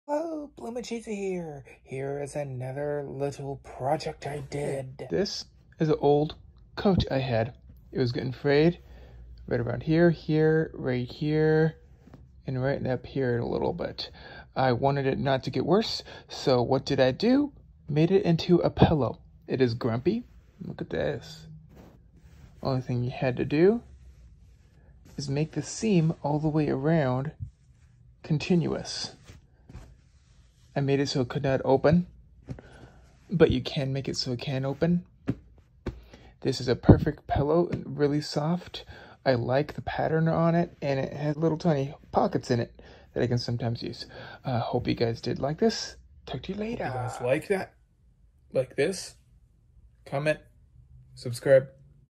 Grumpy coat pillow- day 3 - random Disney vlogmas
Vlogmas day 3- today I show my Christmas coat pillow my sister sewed up for me. Reused coat for many years